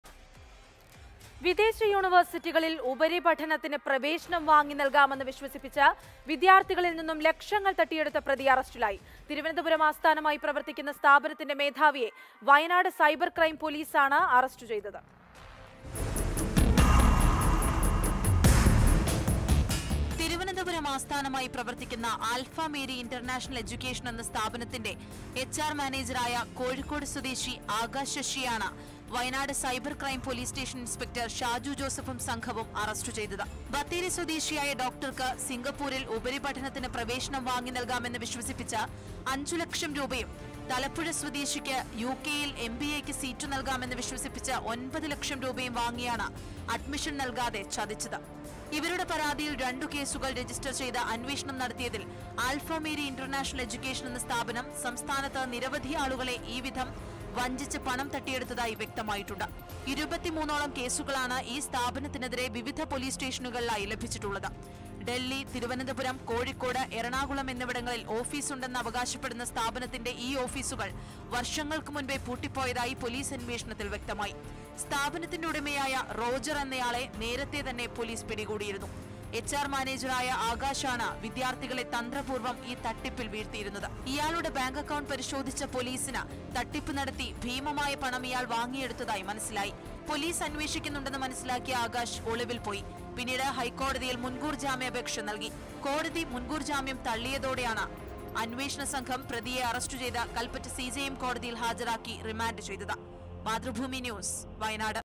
വിദേശ യൂണിവേഴ്സിറ്റികളിൽ പ്രവേശനം വാഗ്ദാനം നൽകി പറ്റിച്ചയാൾ അറസ്റ്റിൽ | Mathrubhumi News
വിദേശ യൂണിവേഴ്സിറ്റികളിൽ ഉപരിപഠനത്തിന് പ്രവേശനം വാങ്ങി നൽകാമെന്ന് വിശ്വസിപ്പിച്ച് വിദ്യാർഥികളിൽ നിന്നും ലക്ഷങ്ങൾ തട്ടിയെടുത്ത പ്രതി അറസ്റ്റിൽ

Happy viewing!